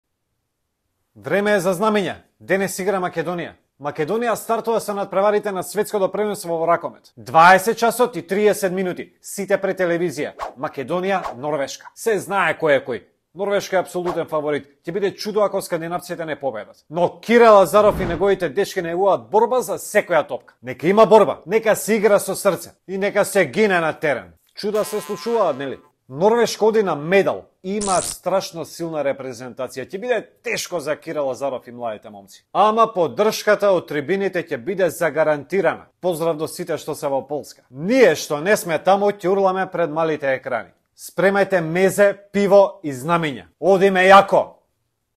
Македонија - Норвешка (ракомет СП 2023)
Македонија стартува на Светското ракометно првенство против Норвешка.
20:30ч. сите пред ТВ!
Се знае кој е кој, Норвешка е огромен фаворит, но Кире Лазаров и момците ветуваат борба на терен ... чуда се случуваат, нели!?